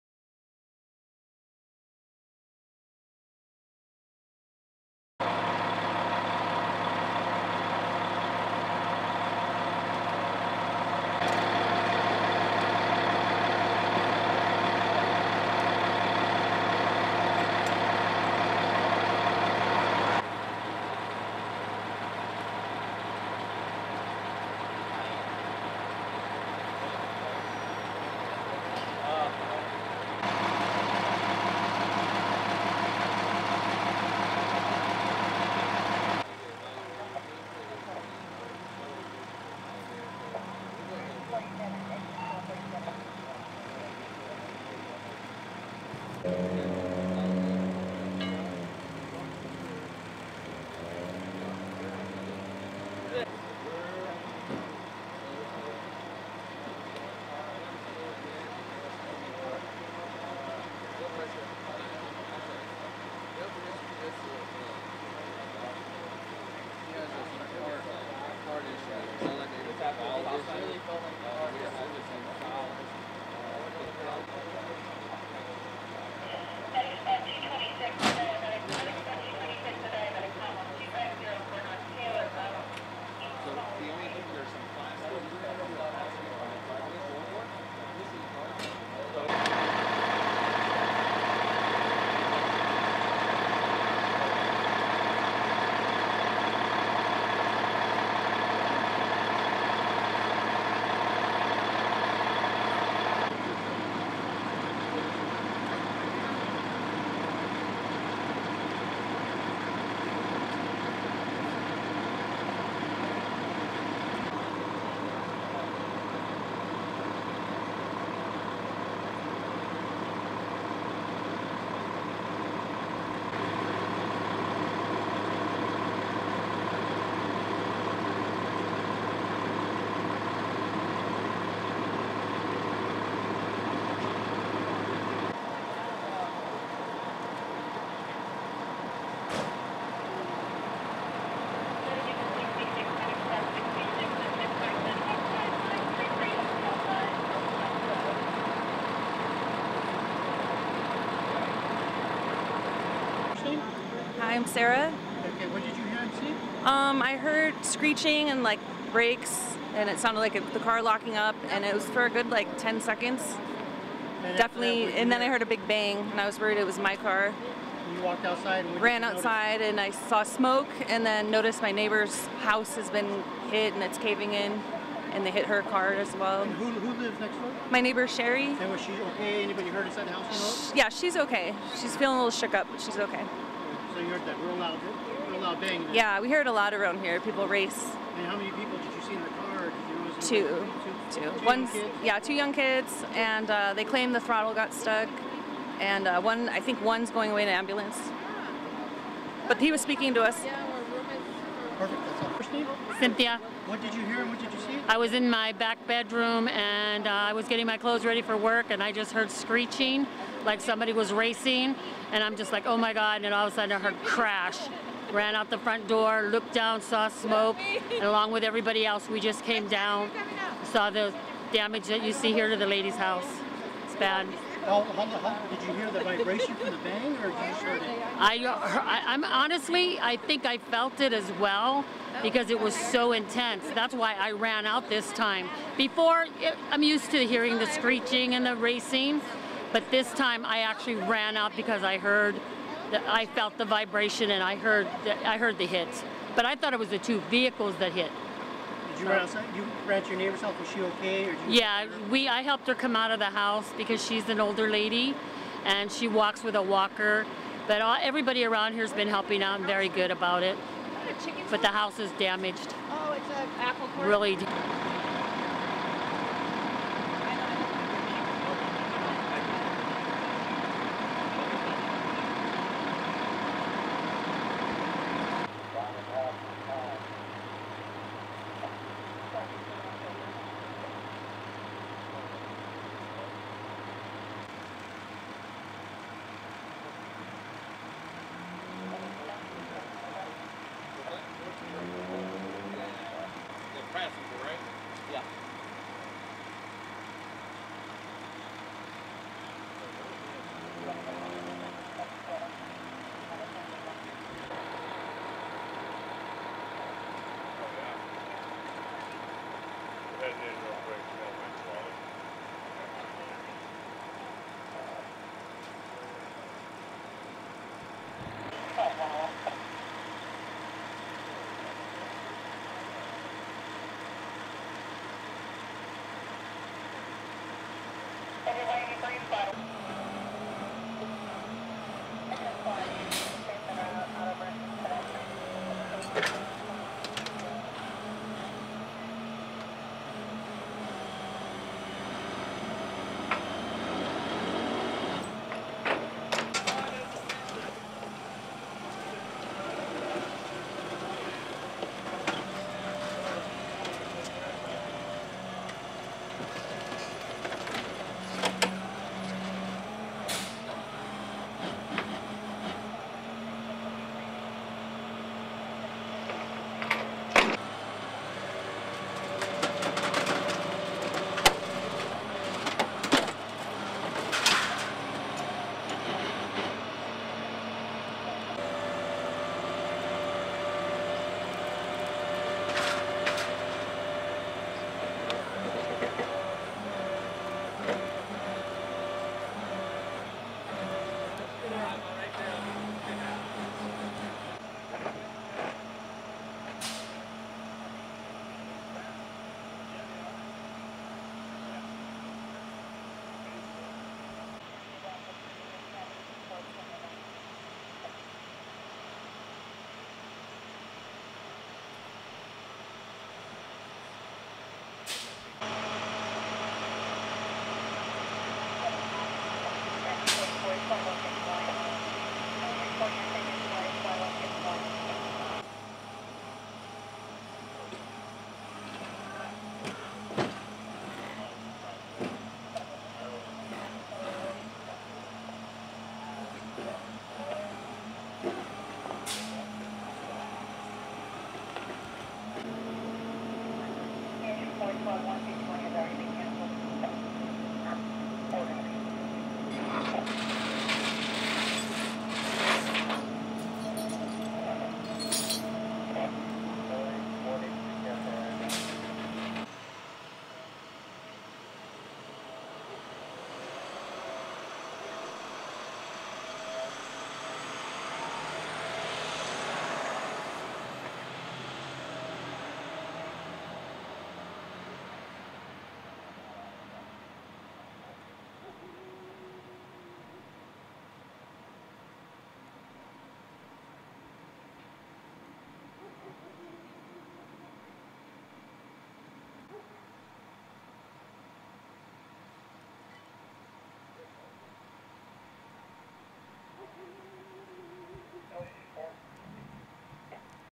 High Speed Car into House / Montclair 10.7.19
MONTCLAIR - At 9:20 pm Montclair Fire and the Montclair Police Department received multiple 911 calls of a high-speed vehicle that crashed into a house at North Benson Ave and W Flora St. Once officers and firefighters arrived on scene they located a 2019 Chevy Camaro in the front yard and into a house causing heavy damage to the structure. Firefighters attended to two young male adults. One was transported to a local hospital with minor injuries and the other male was also transported as a precaution. There was a elderly lady in the house at the time of the accident. Nearby neighbors that heard the accident ran to make sure she was OK and helped her out of the house. The lady was checked out at the scene and cleared with no injuries. A building inspector was called out. Red Cross is on their way out to assist with housing. Nearby neighbor said they heard the car screeching his tire prior to the car crashing into the house. Nearby neighbors said that there have been a lot of street racing going on up and down their street in the past couple of months. Montclair Police Department is handling the investigation into the traffic accident.